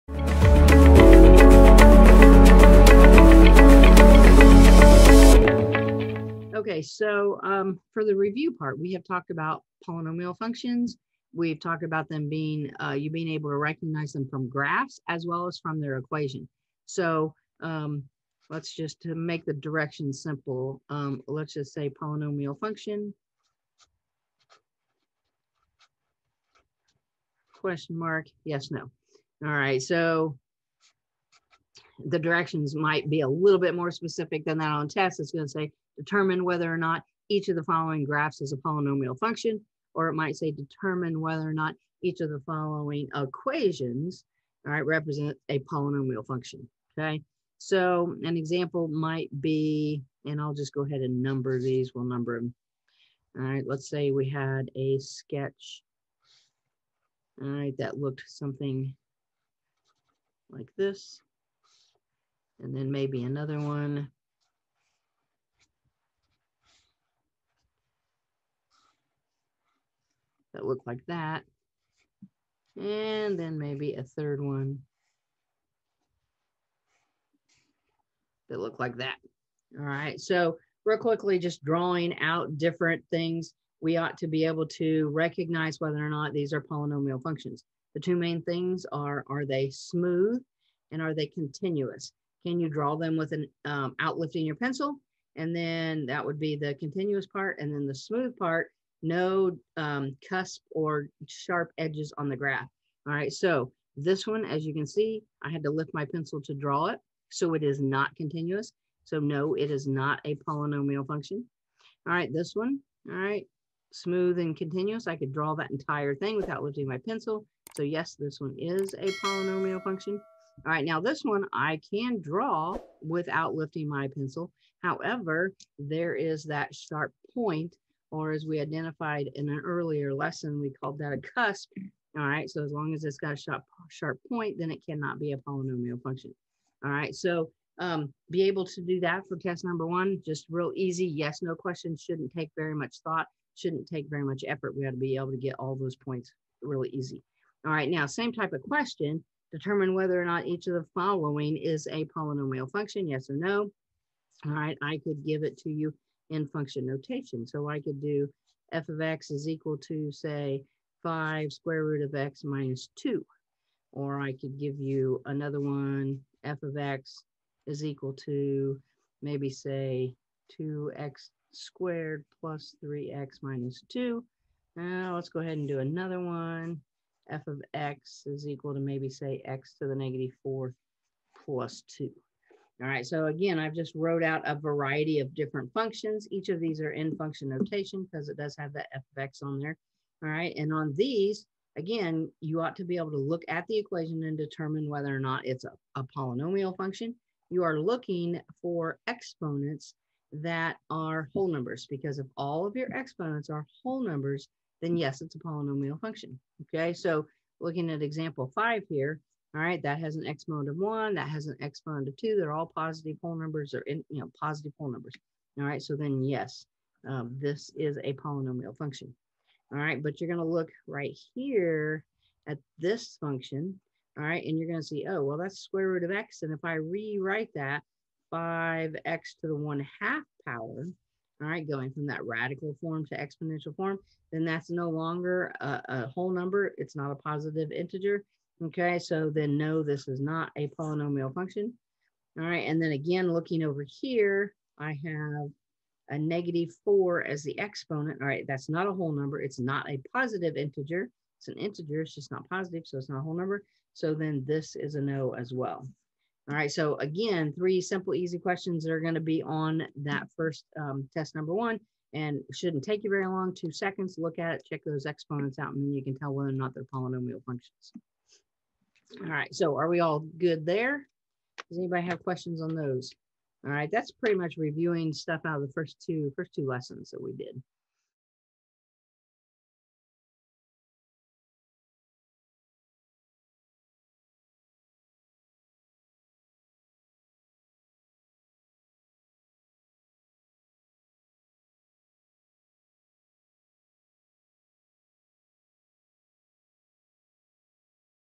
Review of Polynomial Functions
This video goes through a short review of polynomial functions.